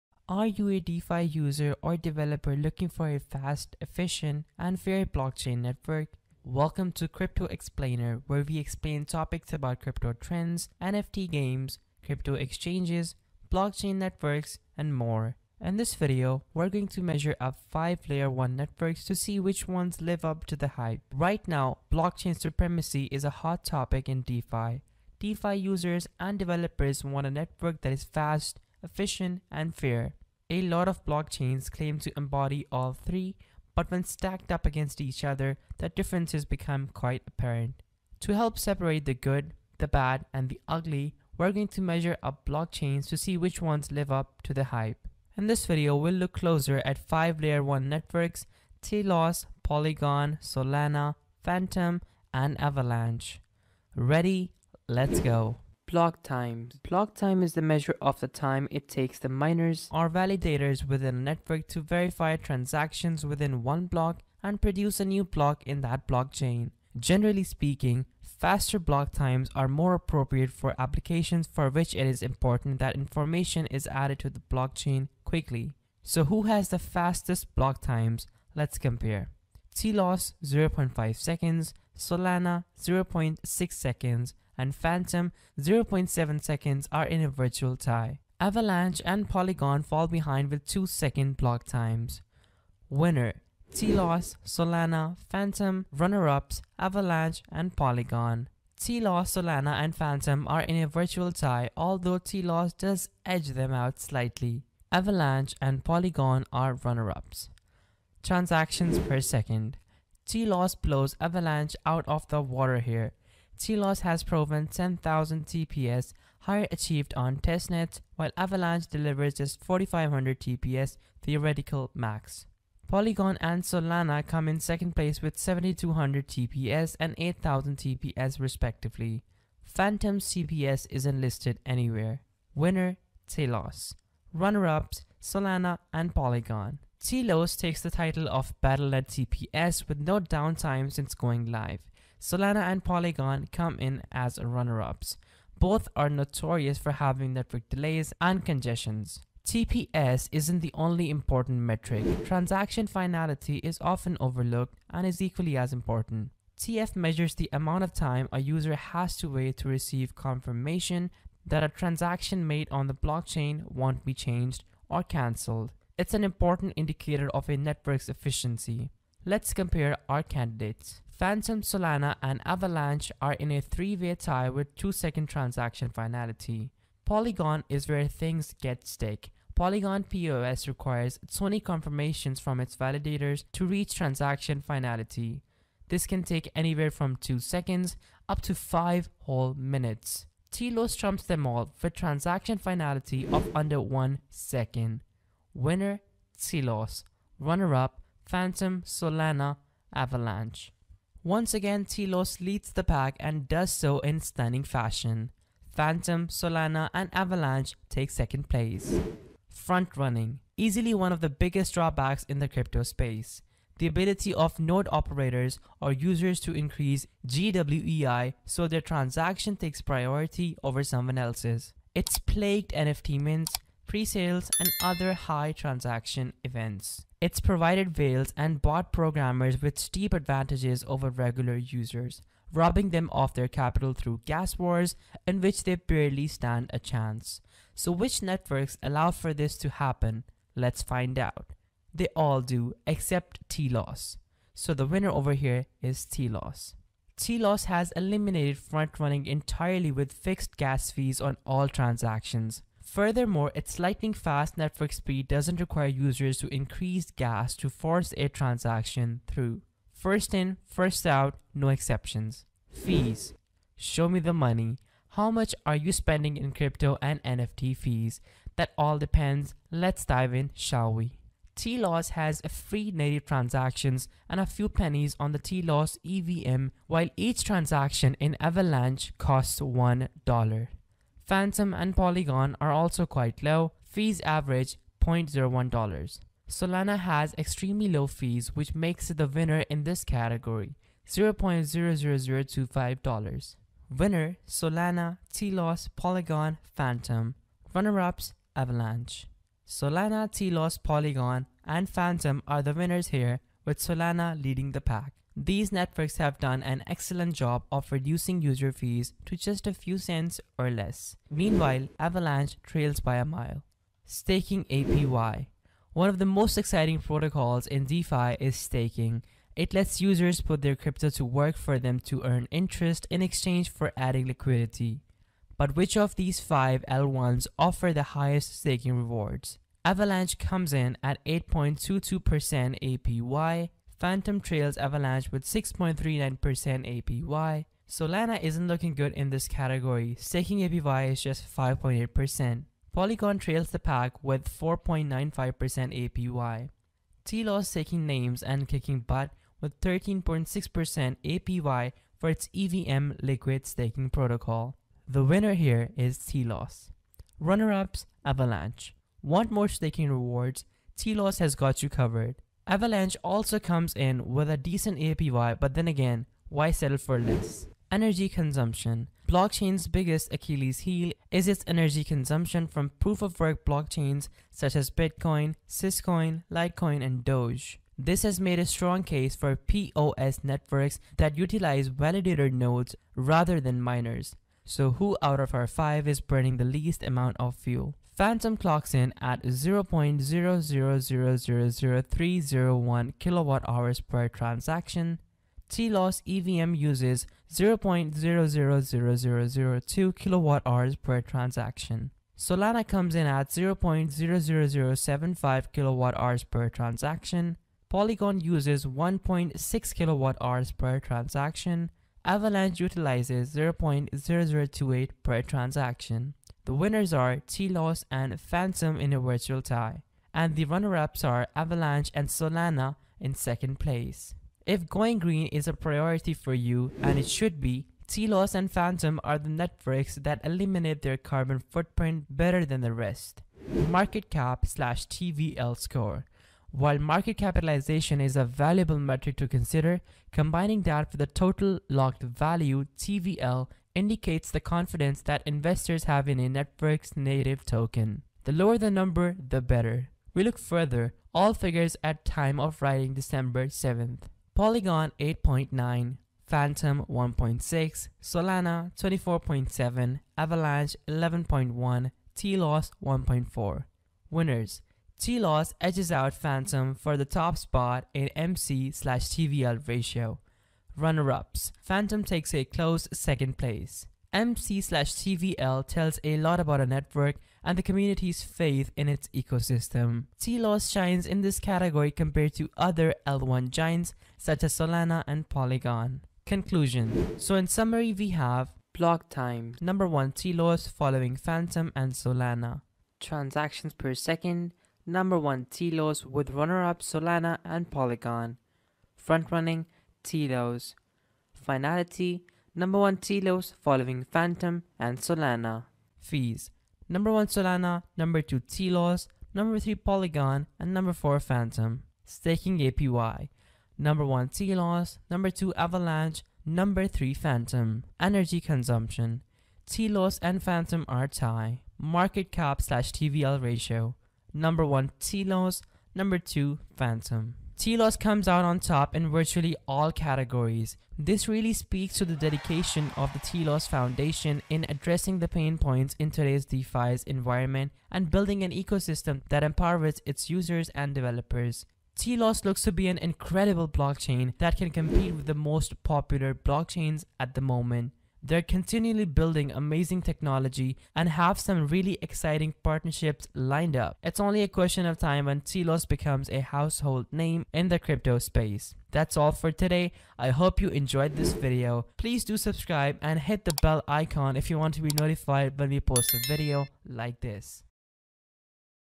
The Blockchain King? Polygon, Solana, Fantom, Avalanche & Telos Blockchain Comparison
Telos Speed | Blockchain Explained | Telos Technical Analysis

Are you a DeFi user or developer looking for a fast, efficient, and fair blockchain network?

Welcome to Crypto Explainer, where we explain topics about crypto trends, NFT Games, Crypto Exchanges,blockchain netowrks and more. In this video, we're going to measure up five Layer-1 Networks to see which ones live up to the hype.

Right now, blockchain supremacy is a hot topic in DeFi. DeFi users and developers want a network that is fast, efficient, and fair. A lot of blockchains claim to embody all three, but when stacked up against each other, the differences become quite apparent.

To help separate the good, the bad, and the ugly, we're going to measure up blockchains to see which ones live up to the hype.

Sit tight, in this video we'll look closer at five Layer-1 networks: Telos, Polygon, Solana, Fantom, and Avalanche. Ready? Let's go!

Keep watching to Learn More about  Polygon, Solana, Fantom, Avalanche & Telos Blockchain Comparison

Best Blockchain, TELOS EVM, Blockchain Network

💎Crypto Explainer💎
So that you will never miss an update on the fast-growing Crypto world.

⏰Timestamps⏰
00:00 Intro
00:56 Blocktimes
01:48 Transactions per Second
02:33 Transaction Finality
03:30 Front Running
04:24 Fees
05:16 Staking APY
06:08 Energy Consumption
07:13 Market Cap/TVL Score
08:07 Conclusion